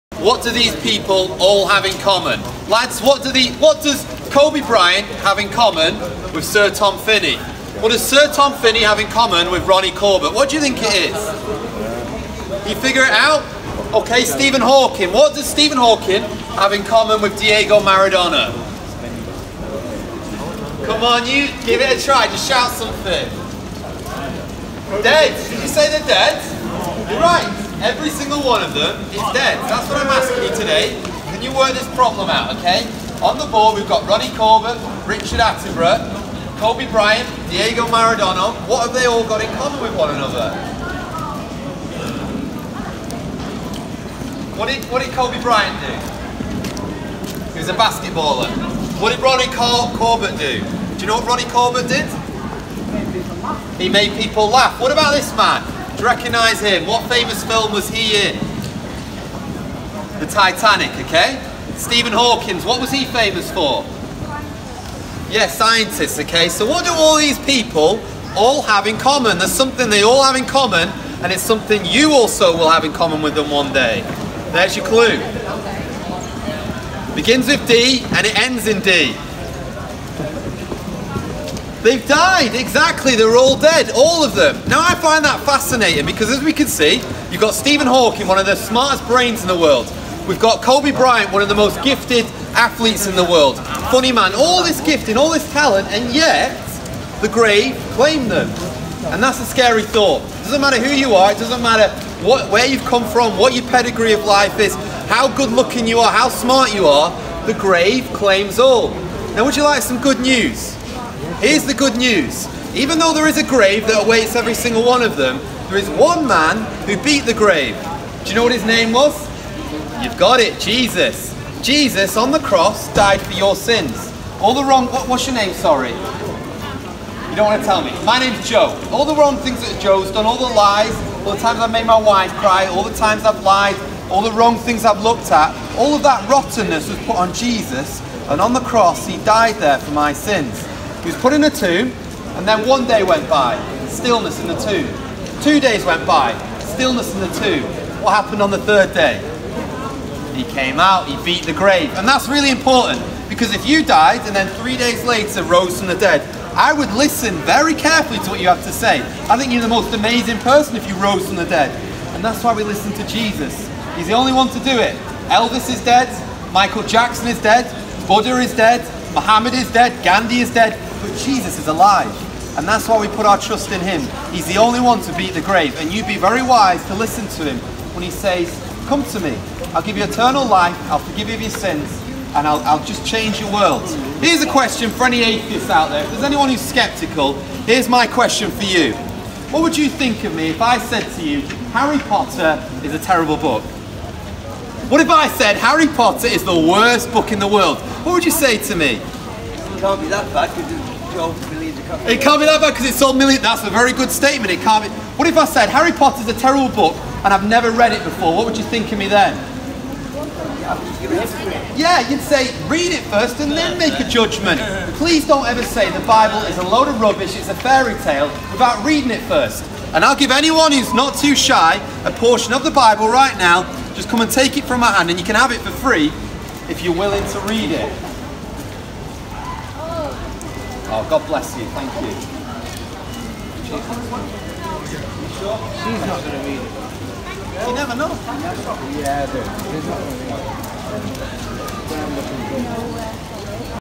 Preacher Stumps People With This Question...
Street preacher Joe Kirby on celebrities whilst open air preaching in Lancaster England. @offthekirb